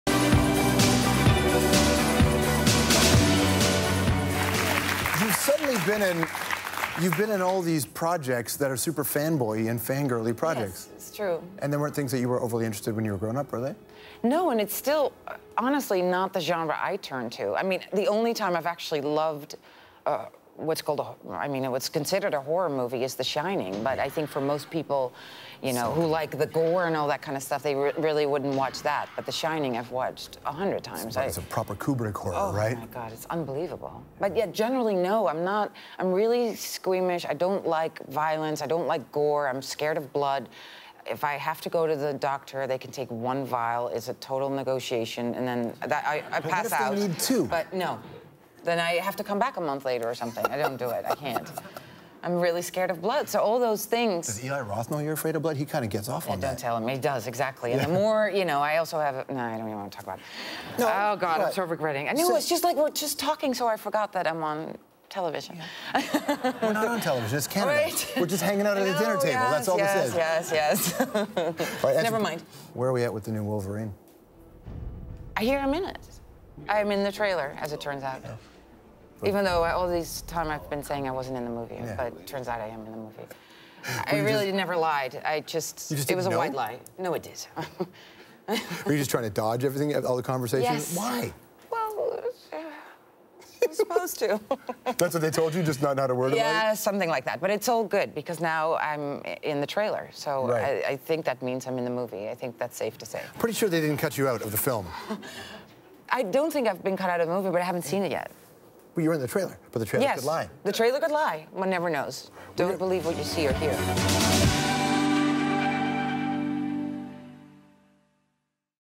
Famke Janssen Talks 'Hemlock Grove' And (Maybe?) Being In 'The Wolverine'
Fan favourite, Famke Janssen, recently sat down with George to talk about her new role on Hemlock Grove. If you're a horror fan, and can stomach the grisly scenes, then tune in on April 19 -- when all 13 episodes will be released exclusively to Netflix.

In this clip, Famke reveals that she's not exactly a fan of blood... but don't tell that to Hemlock Grove maker, Eli Roth.

We also hear the latest on her involvement with The Wolverine.